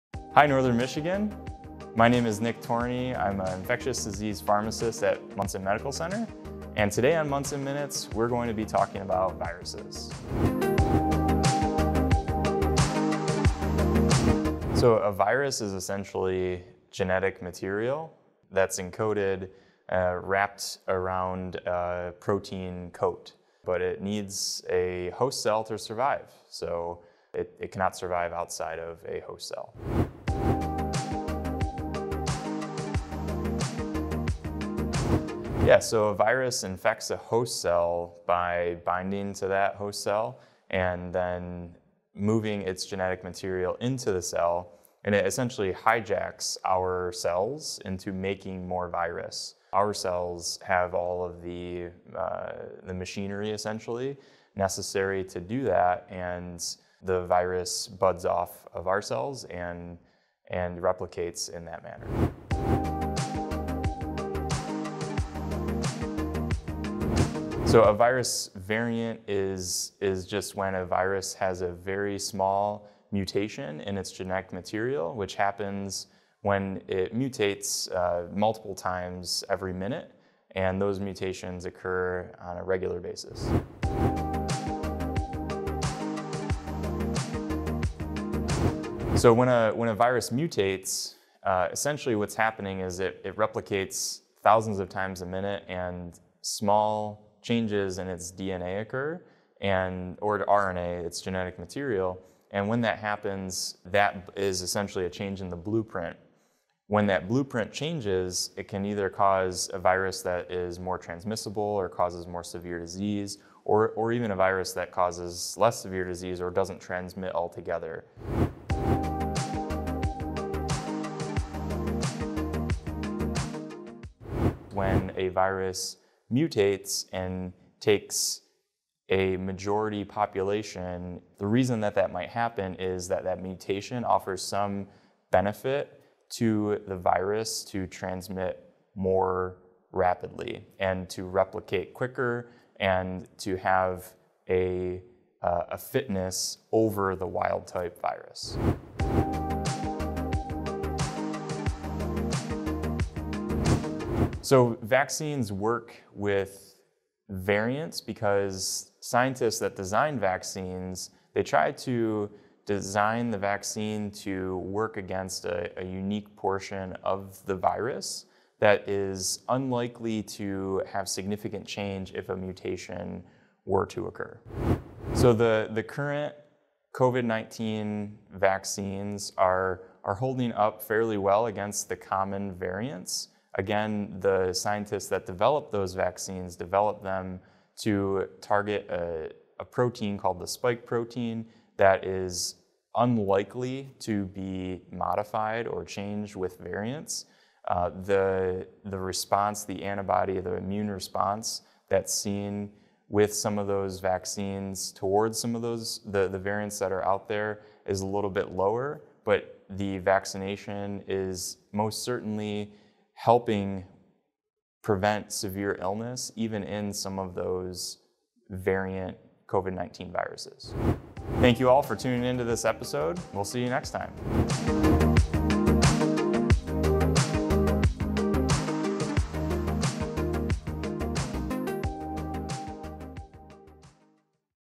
Viruses: Variants and Mutations | Munson Minutes
It's Virus Variants 101! How do virus variants and mutations form? Why do virus variants seem to spread faster than the original virus? And perhaps most importantly, how are the COVID-19 vaccines holding up against these new virus strains? Munson Healthcare's Nick Torney, PharmD, BCIDP, Infectious Diseases breaks it all down in this episode of Munson Minutes.

Nurses are available daily from 7 am - 11 pm at no charge.